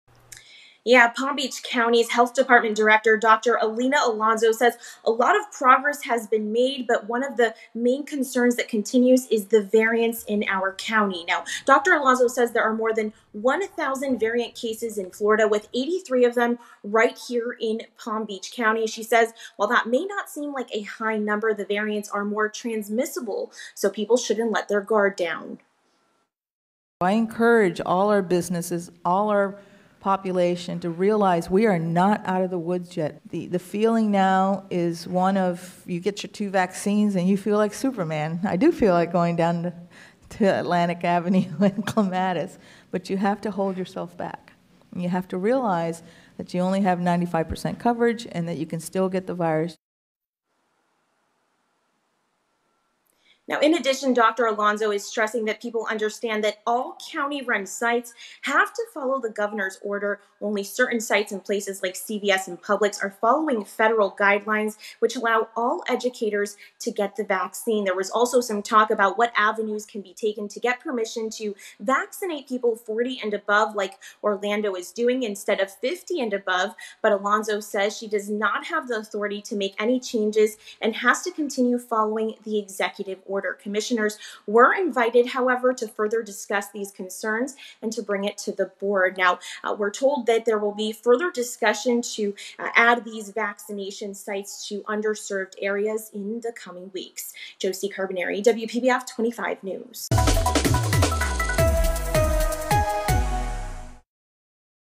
Concern about COVID-19 variants in Palm Beach County
Dr. Alina Alonso said Tuesday that we are not out of the woods yet when it comes to COVID-19. WPBF 25's Jossie Carbonare explains.